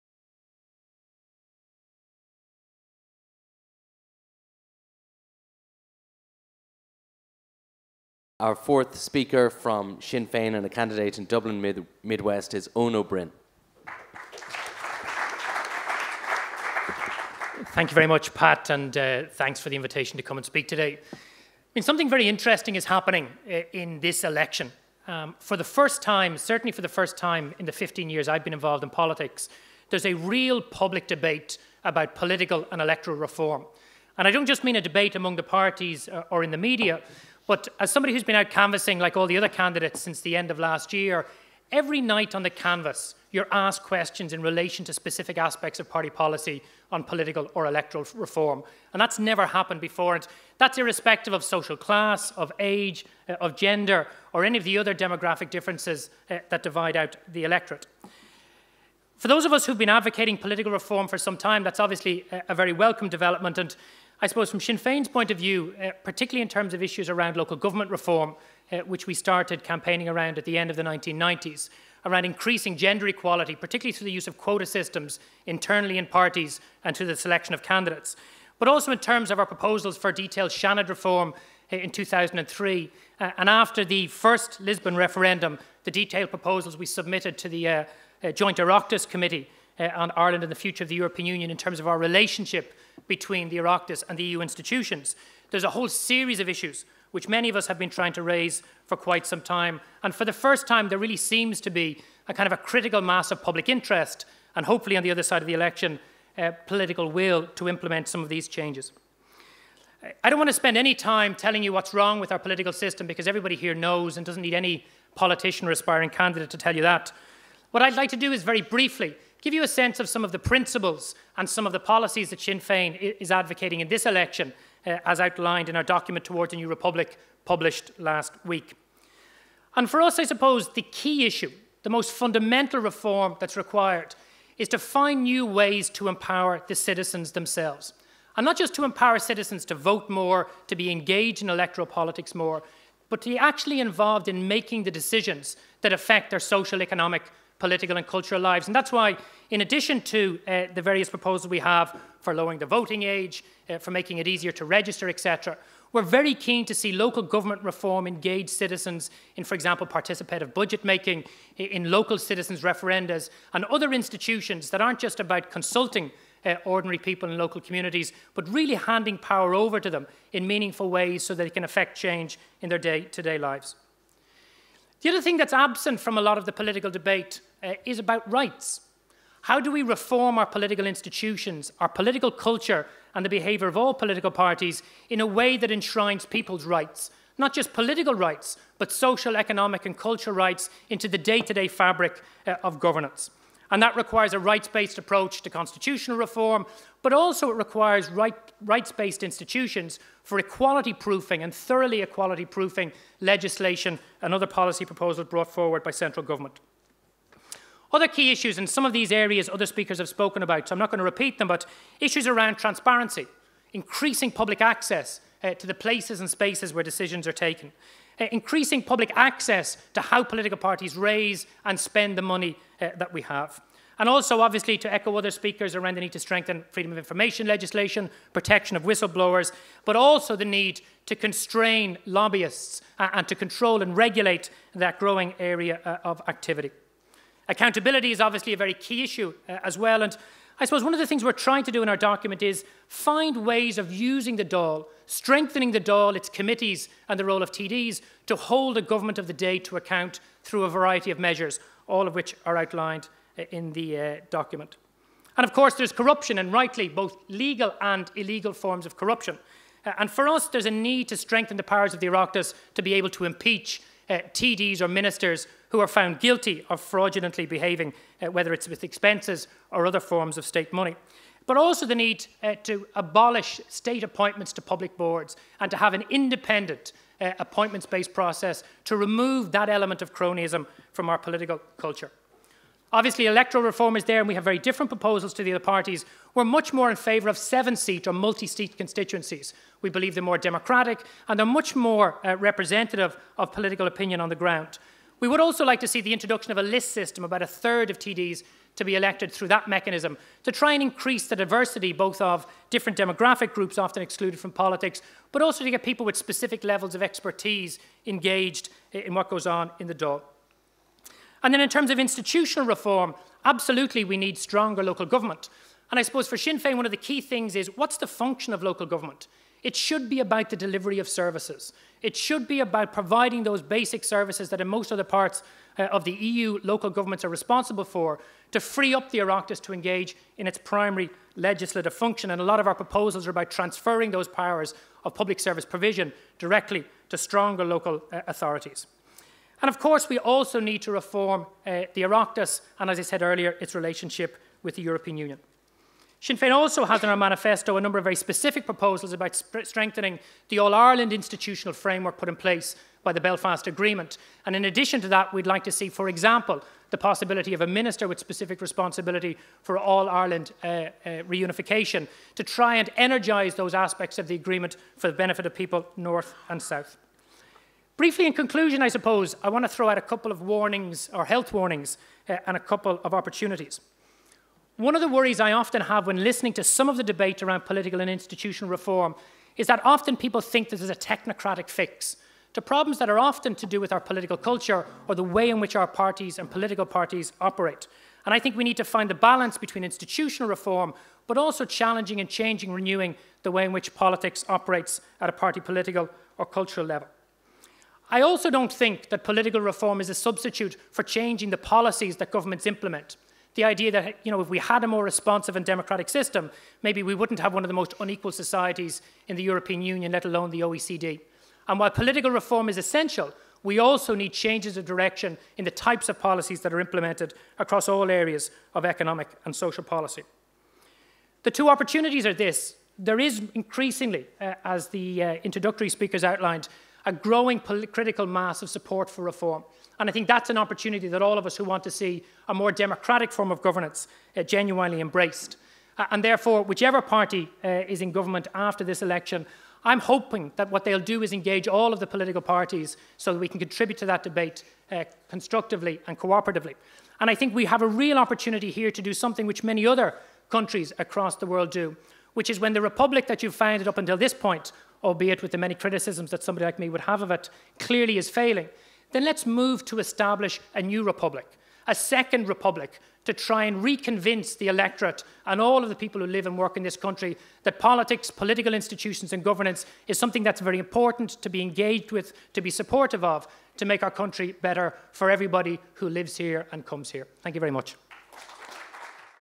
5. Real Reform: Election Score Card 2011 - Eoin O Broin, Sinn Féin
Eoin O Broin speaks at a a free public public forum on the political parties' proposals for reform during Election 2011.

This event was hosted with ReformCard.com in The Sugar Club at 1.00pm on Monday 21 February. It was chaired by Pat Leahy, Political Editor of the Sunday Business Post.

ReformCard.com scores of each party's proposals in five key areas: Legislative; Electoral; Open government; Local government; and Public sector.